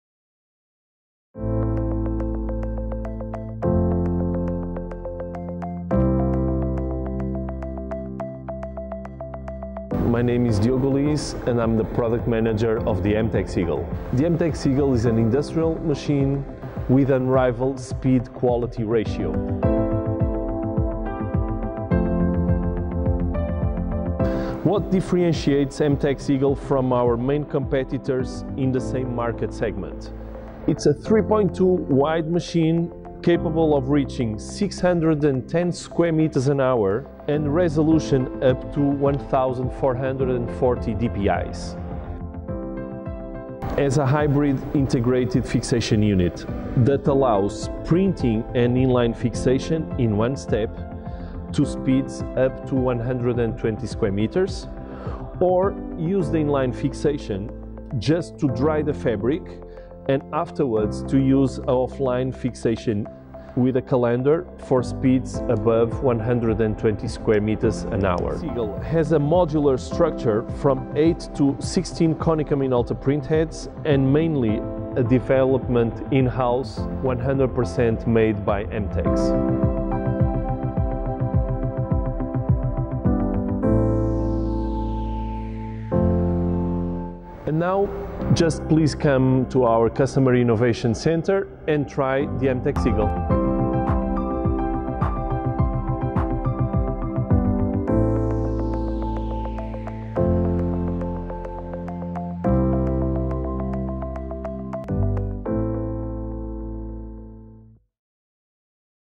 Meet Diogo Liz - Product Manager for MTEX EAGLE
The MTEX EAGLE proposes to the market an industrial high-speed printer, specifically engineered for direct fabric sublimation. At production mode – 720 DPI, the Eagle can print 304 sqm/h thanks to its advanced mechanical and electronic structure. The Eagle features innovative MTEX Technology and an inbuilt fixation unit that ensures exceptionally high quality, vibrant colors and consistent output at a high-speed printing onto polyester.

Depending on the polyester type you can do in one-shot:

Up to 120 sqm/hr: printing and inline fixation;
Above 120 sqm/hr: using inline fixation as a dryer and afterwards fixate using an external calender.
16 Konica Minolta printheads
Magnetic Linear motor

Printing width: 330cm
Resolution: from 360 up to 1440 DPI
Drop size: 4 picolitres (high speed), low ink consumption
Media feed (Entry): standard roll unwinder up to ø 400 mm for 330 cm
Media take-up (Exit): standard roll winder up to ø 400 mm for 330 cm
Inline Fixation Unit: For speed up to 120sqm/h